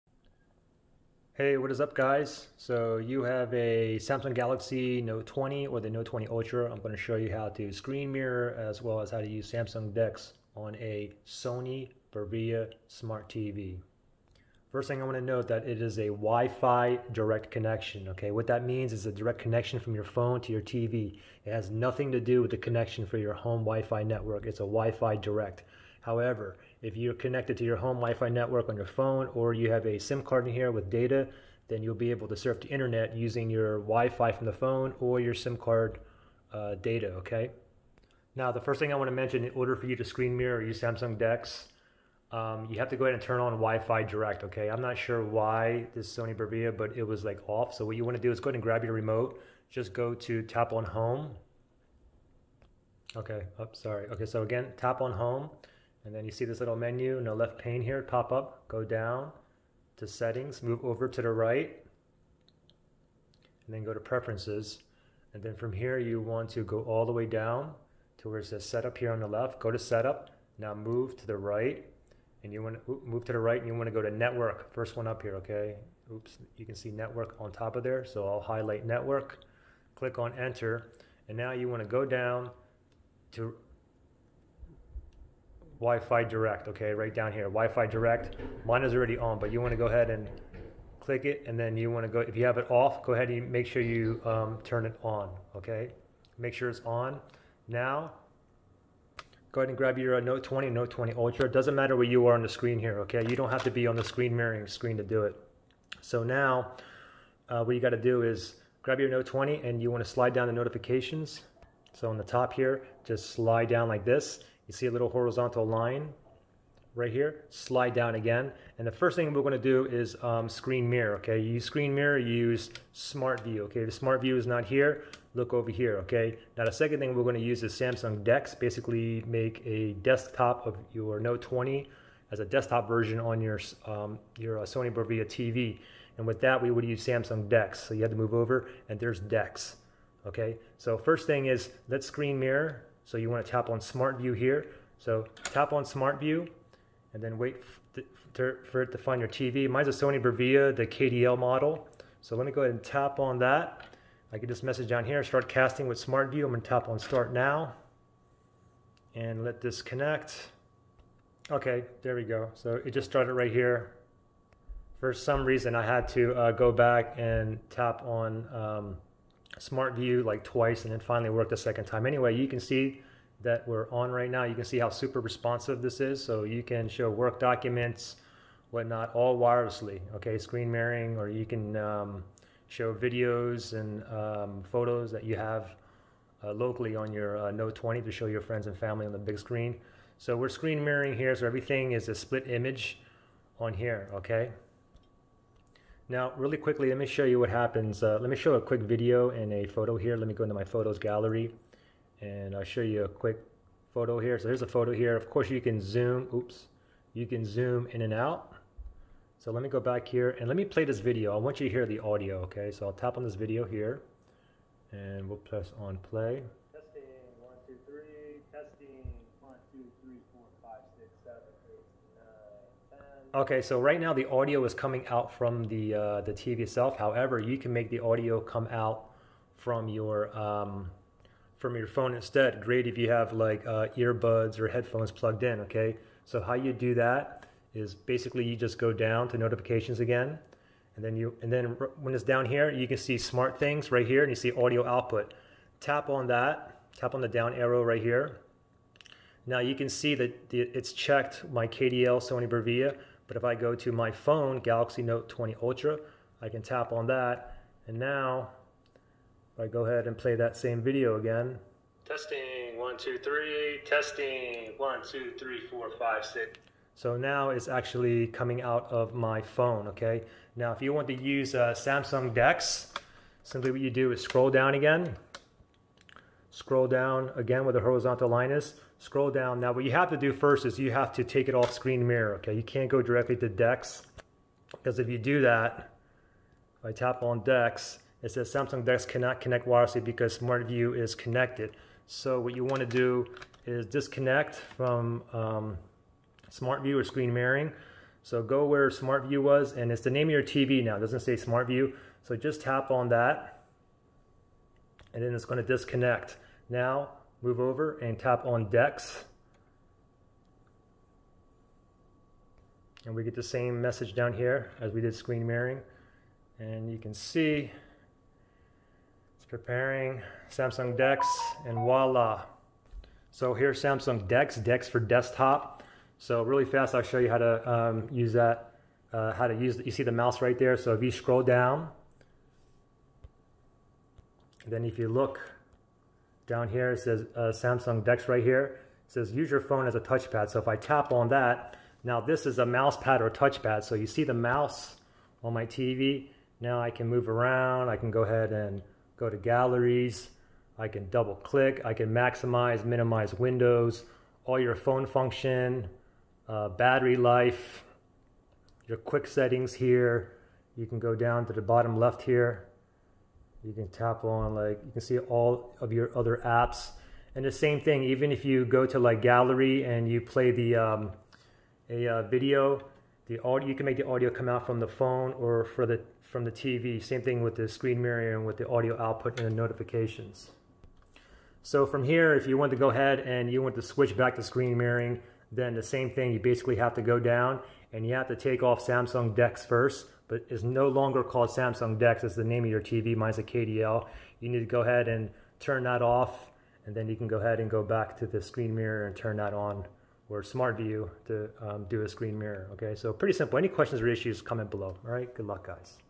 Galaxy Note 20: Wireless Screen Mirror & Samsung DEX on Sony Bravia Smart TV
I show you connect wireless screen mirror and Samsung DEX from a Samsung Galaxy Note 20 or Note 20 Ultra to a Sony Bravia smart TV (non Android TV). Hope this helps.

Galaxy Note 20 / Note 20 Ultra unlocked
Sony Bravia KDL32W600D 32-Inch HD Smart TV